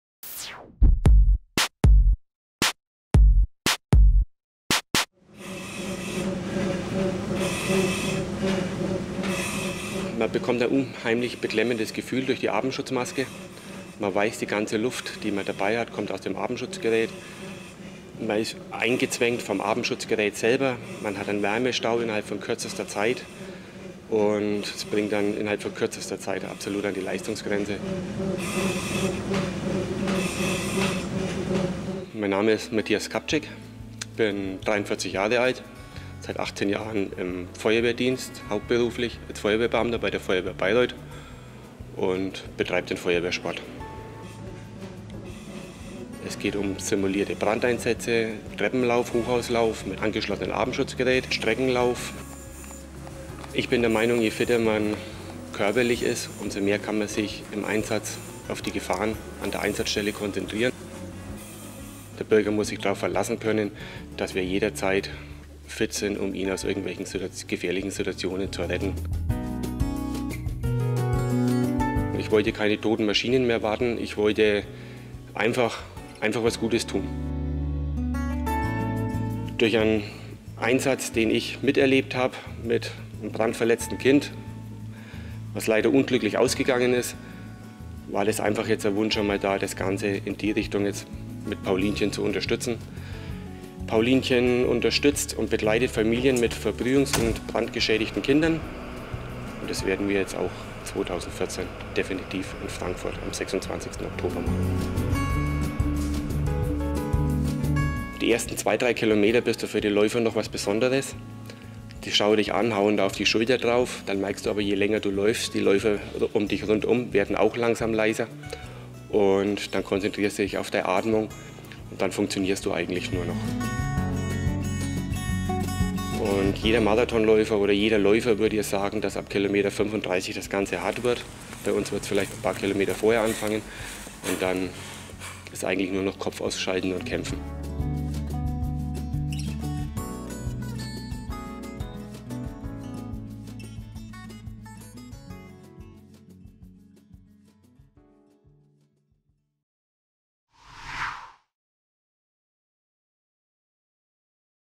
Bayreuther Feuerwehrmann läuft für Brandopfer
Matthias Skapczyk (43) läuft Marathon. Er trägt dazu Feuerwehranzug und Atemschutz. Skapczyk will auffallen. Es geht ihm nicht um Höchstleistungen, sondern um die gute Sache. Der Feuerwehrmann sammelt Spenden für die Initiative Paulichen, die sich um brandverletzte Kinder kümmert. Jetzt startet Skapczyk beim Frankfurt-Marathon.